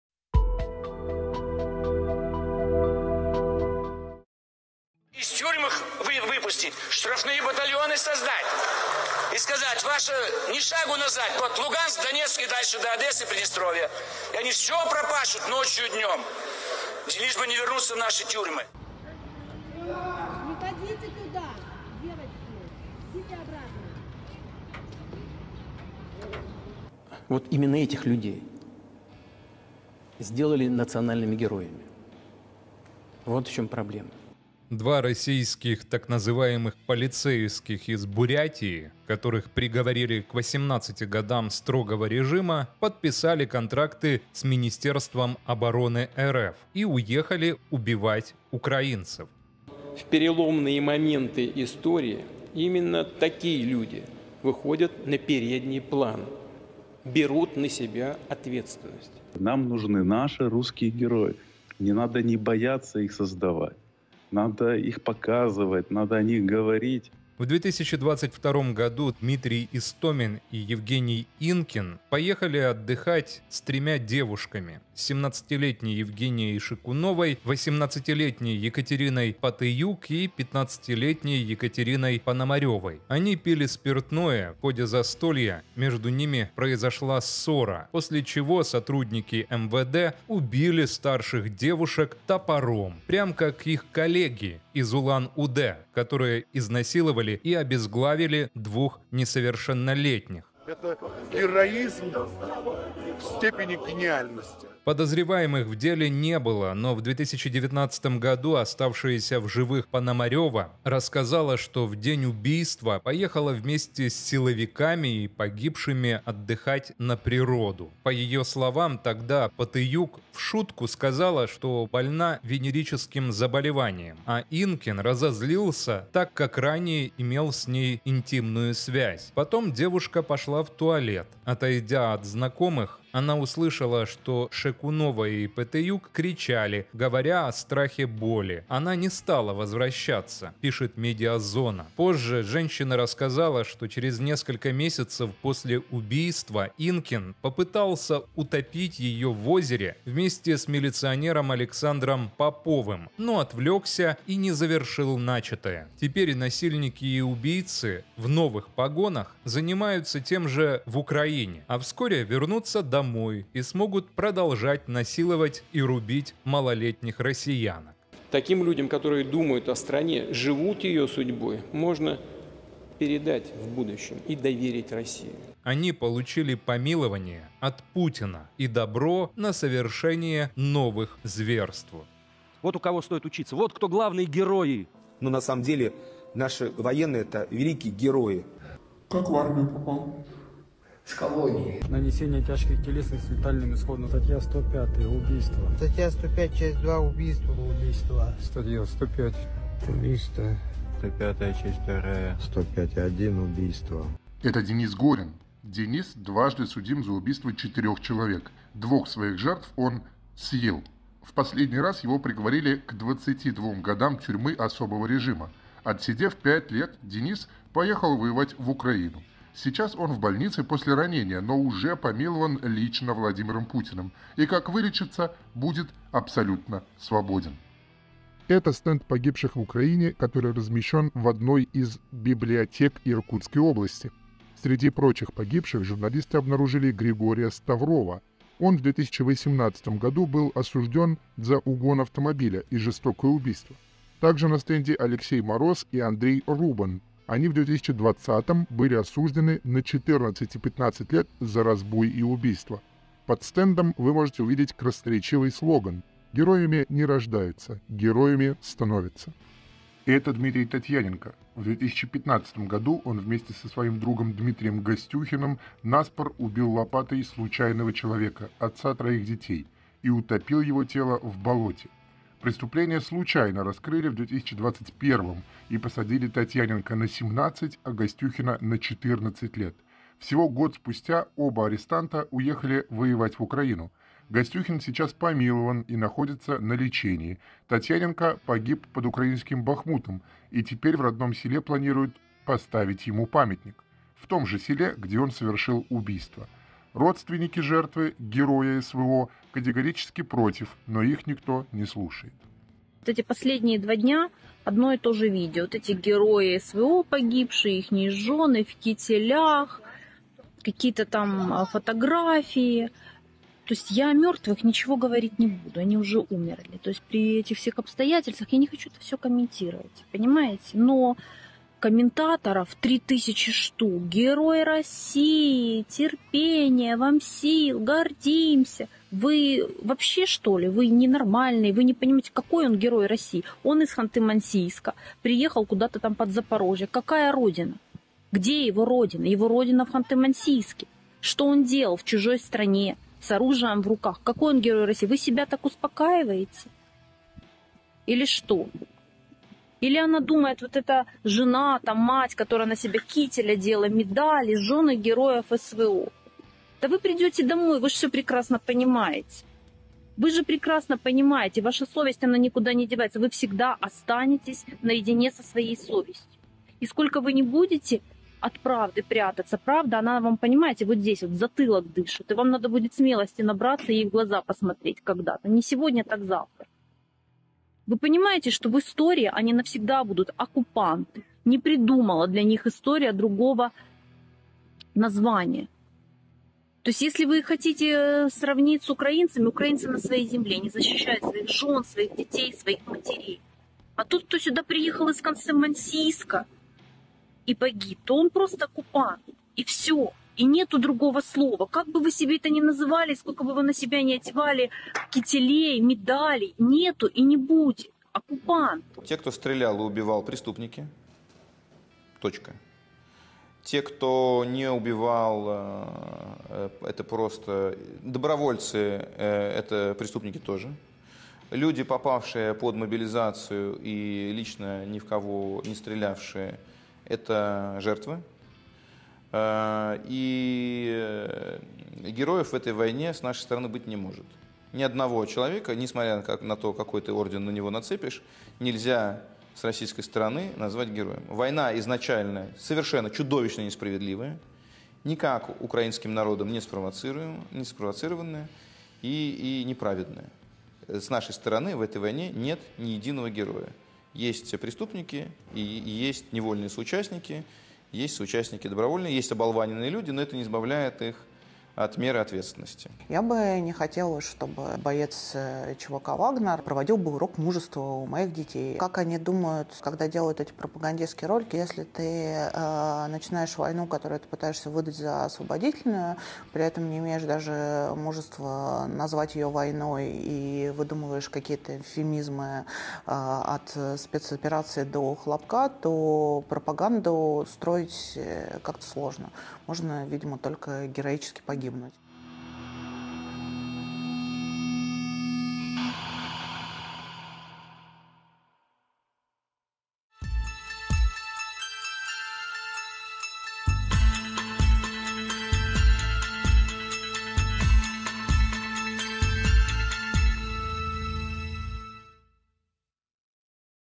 Новая элита россии? Кого путин помиловал и благословил на новые "подвиги"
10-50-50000 - будь-яка сума має значення.

Разом переможемо!
Слава Україні!